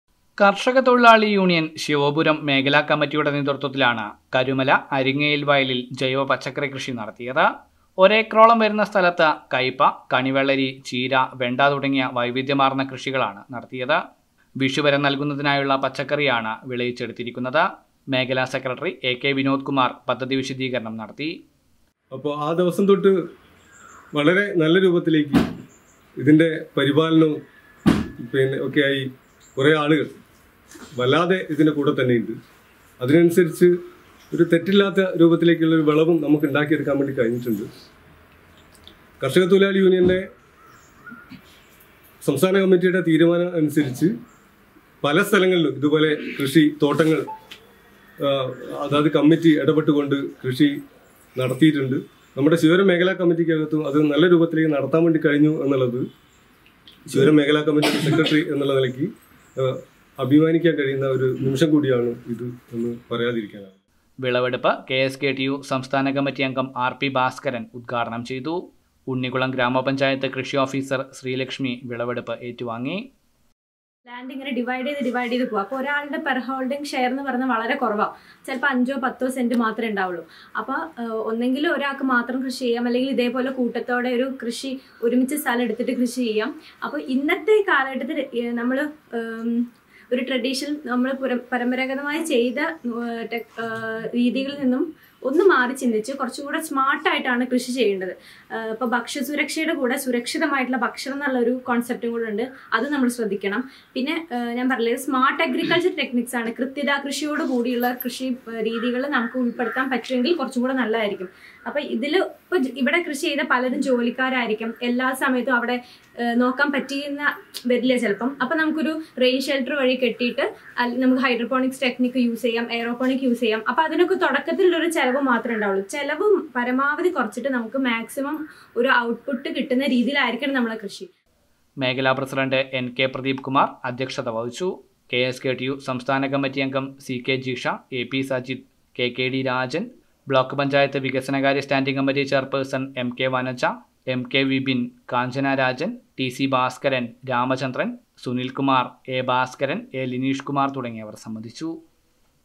കെ എസ് കെ ടി യു ബാലുശ്ശേരി ശിവപുരം മേഖല കമ്മിറ്റി നടത്തിയ ജൈവ പച്ചക്കറി കൃഷിയുടെ വിളവെടുപ്പ്
കർഷക തൊഴിലാളി യൂണിയൻ കെ എസ് കെ ടി യു ബാലുശ്ശേരി ശിവപുരം മേഖല കമ്മിറ്റി കരുമല അരിങ്ങയിൽ വയലിൽ നടത്തിയ ജൈവ പച്ചക്കറി കൃഷിയുടെ വിളവെടുപ്പ് നടന്നു.യൂണിയൻ സംസ്ഥാന കമ്മിറ്റി അംഗം ആർ പി ഭാസ്കരൻ ഉദ്ഘാടനം ചെയ്തു